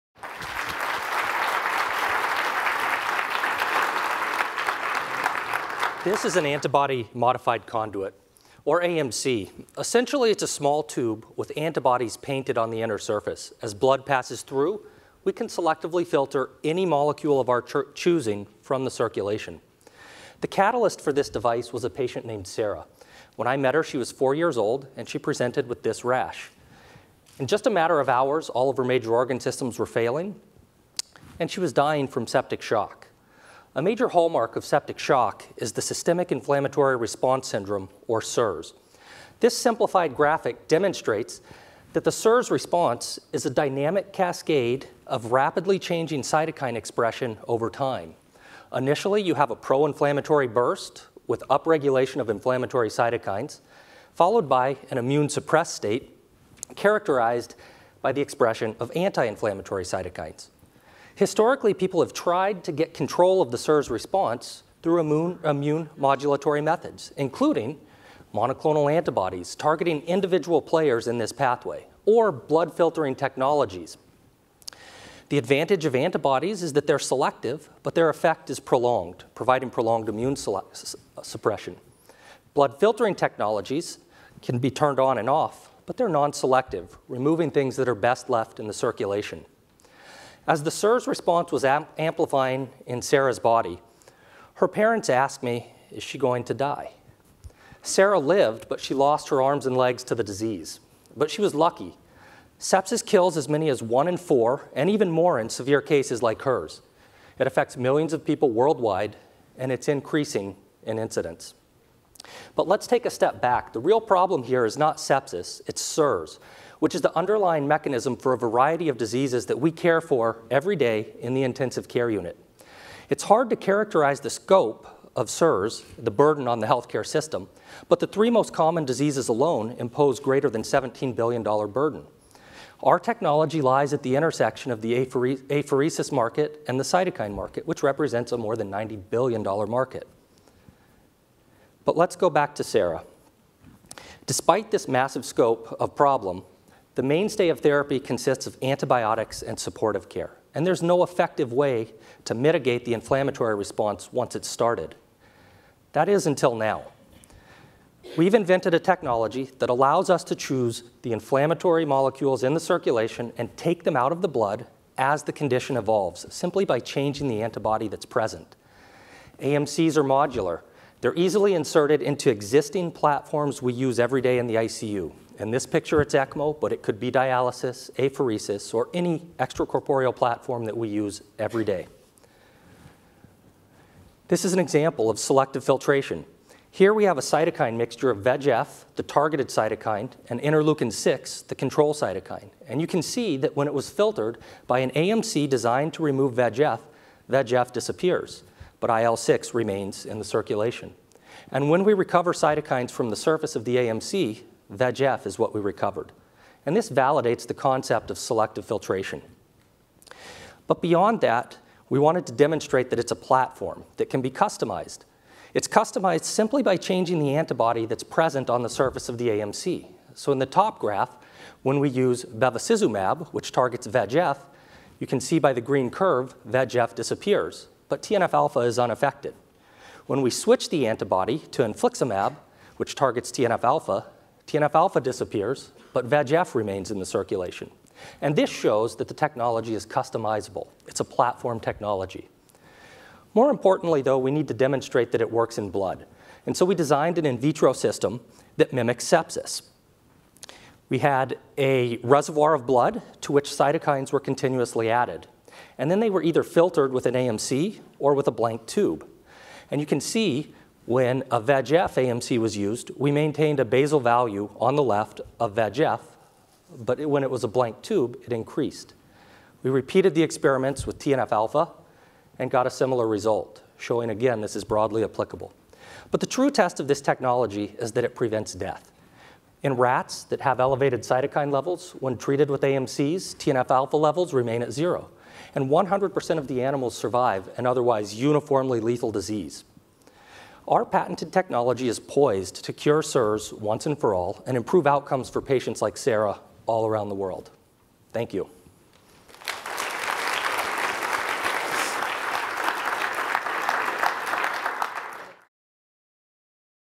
Ignite Talk: Brian McAlvin | Boston Children’s Hospital Global Pediatric Innovation Summit + Awards
Sepsis affects millions of people worldwide and is increasing in incidence, yet there’s no effective way to mitigate the inflammatory response once it’s started. At the 2015 Boston Children’s Hospital Global Pediatric Innovation Summit + Awards, Brian McAlvin, MD, a critical care intensivist at Boston Children’s Hospital, introduces the audience to a technology called an Antibody Modified Conduit (AMC). By filtering inflammatory molecules from the circulation, AMCs can cure SIRS, the underlying mechanism for a variety of diseases including sepsis.